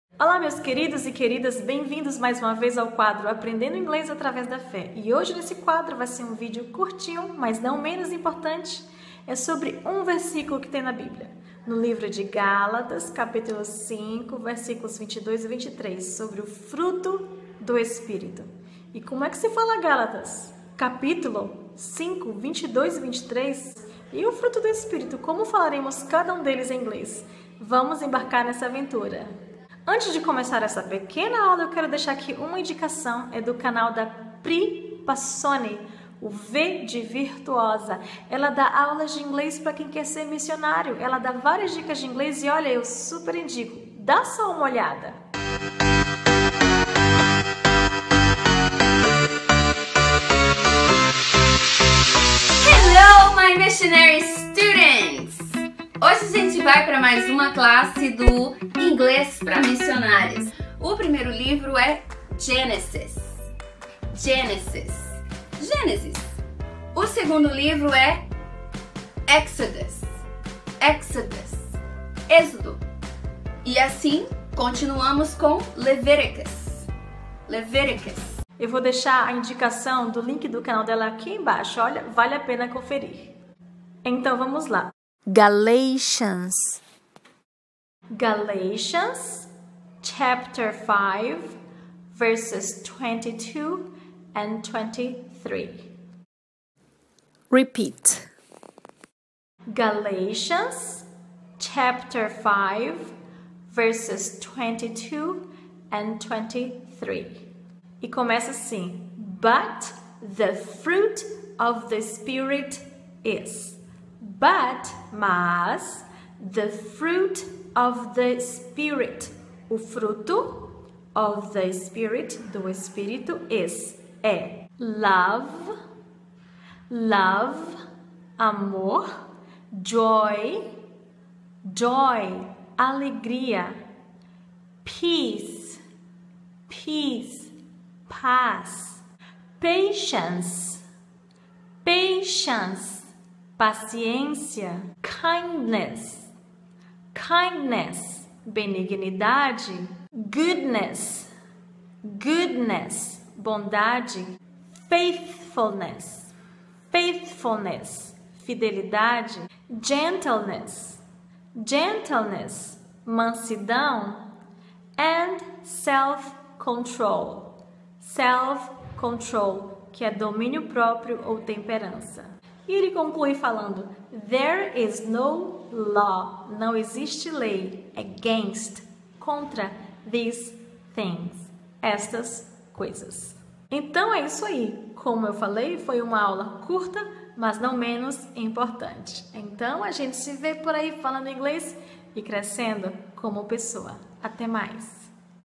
O Fruto do Espírito em Inglês - Aprendendo através da Fé
E que tala prender a dizer isso em inglês?
"Mas o Fruto do Espírito é:
Amor, Alegria, Paz, Paciência, bondade, benignidade, fidelidade, Mansidão e Domínio Próprio". Gálatas 5:22,23